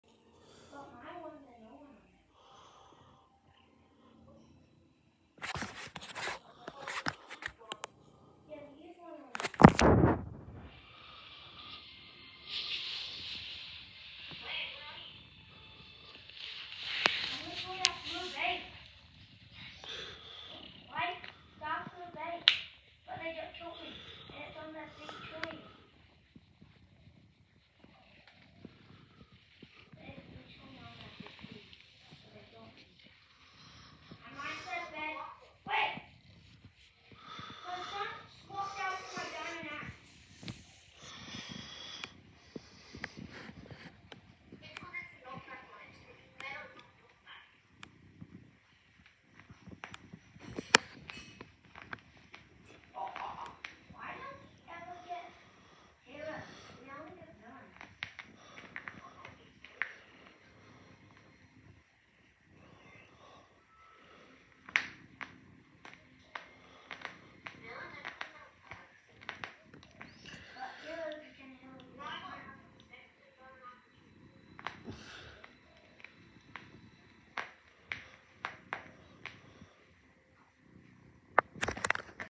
Hey everyone so this is my Grandma’s cat Puda, today he is happy because I pat him and he purrs.
Grandma cat Puda he is happy today